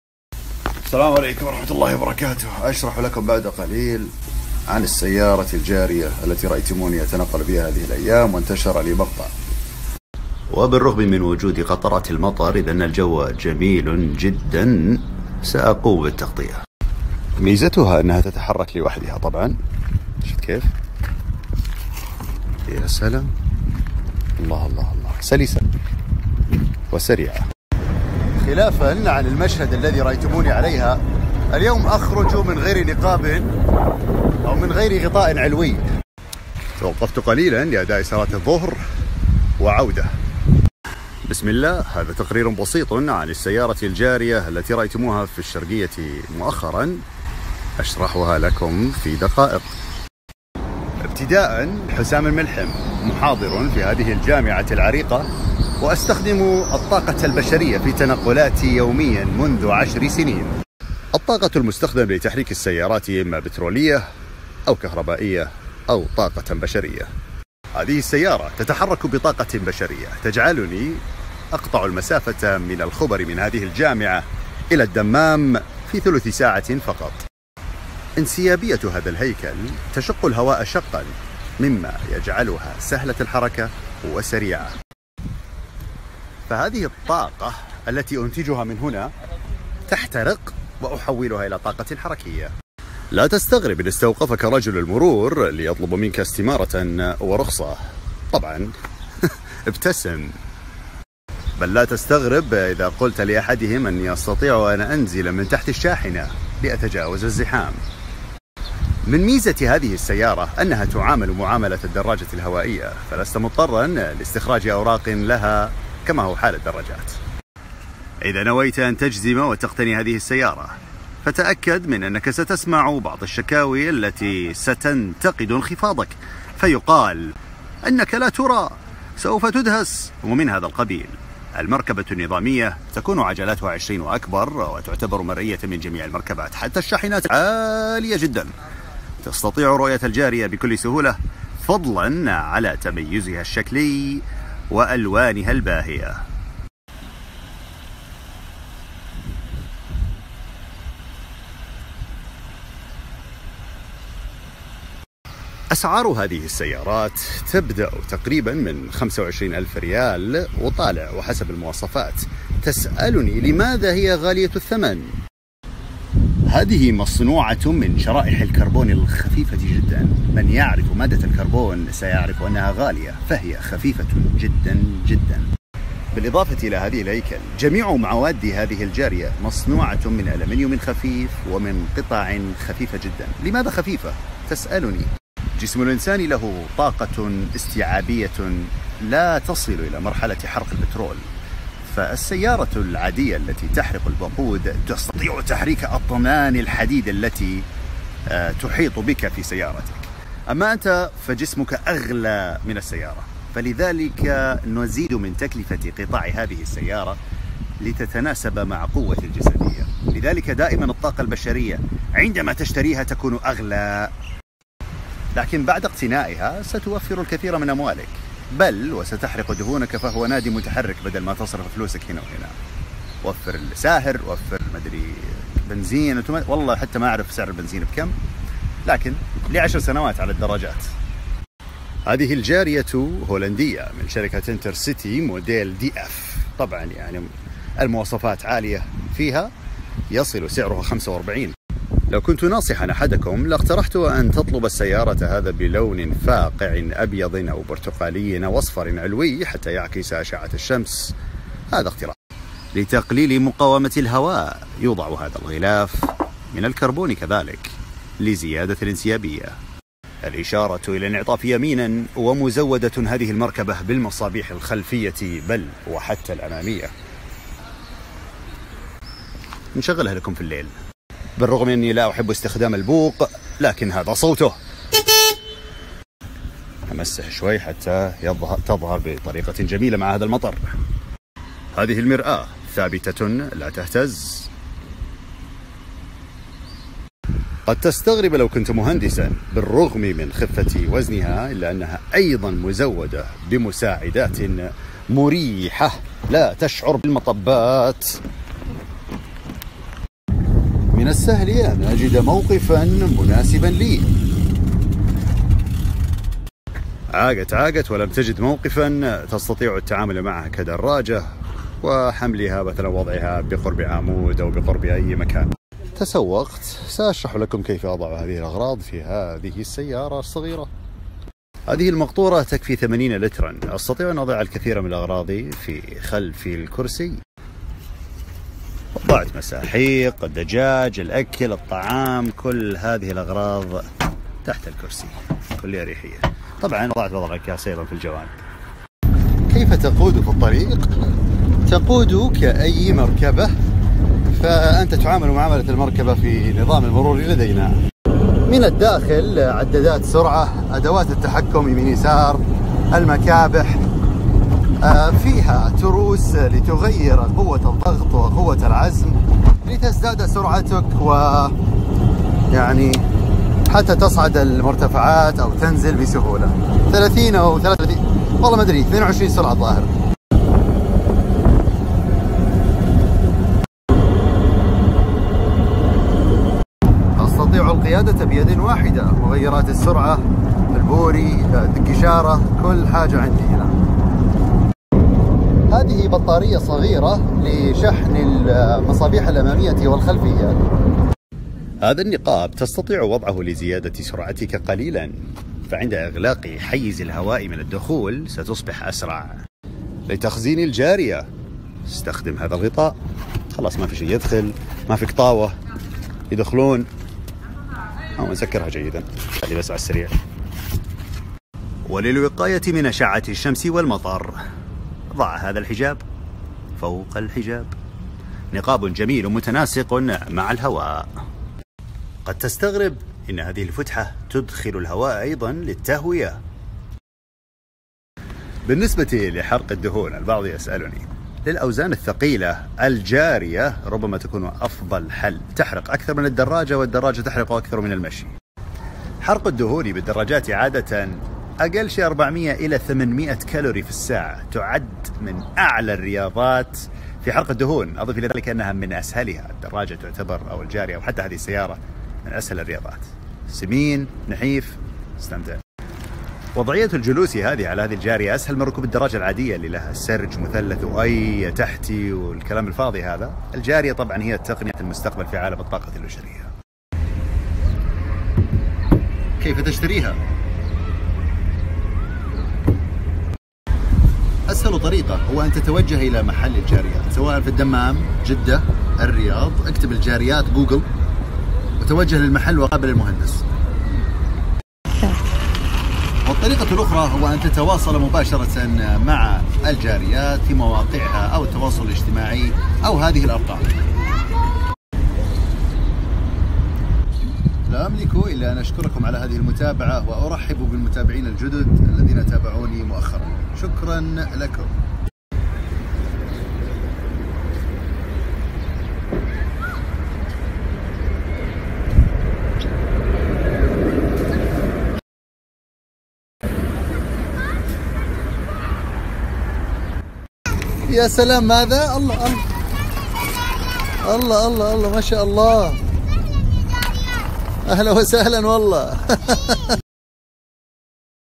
تقرير أول سيارة جارية
وصول أول سيارة تسير بالطاقة البشرية، جارية محجبة صناعة هولندية، التقرير من حسام الملحم.

الوكيل المتخصص في دراجات وجاريات هوائية من شركات برفورمر و كاترايك و آيس و تيرا_ترايك و